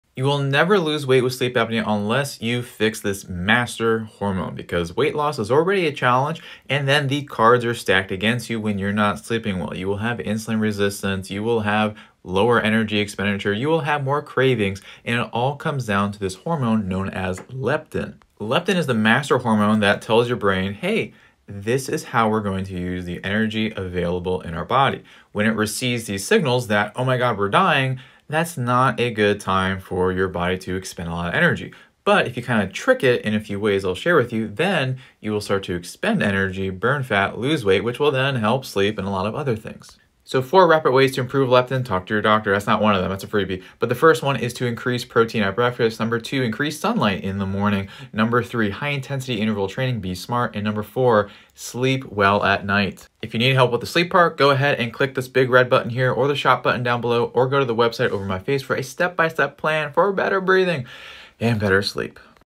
You’ll never lose weight with sleep apnea — unless you fix this master hormone first
(Tap @apneareset to get your clear, personalized path to better sleep)

That hormone is leptin — your body’s main energy regulator.
It tells your brain how much energy you have and how freely you can burn it.

When you’re sleep-deprived, your brain reads that as starvation.
Leptin signals get scrambled.
Your body holds onto fat, lowers energy output, and ramps up cravings — the perfect storm for weight gain.

But the good news: you can reset leptin sensitivity fast.

1️⃣ Eat protein-heavy breakfasts to stabilize energy early.
2️⃣ Get morning sunlight to reset your circadian rhythm.
3️⃣ Add short bursts of HIIT (with medical clearance).
4️⃣ And of course — fix your sleep so your hormones can normalize.

This is why weight loss feels impossible until your sleep and breathing are dialed in.

That’s a good quick fix — but the real results come when your plan matches your body.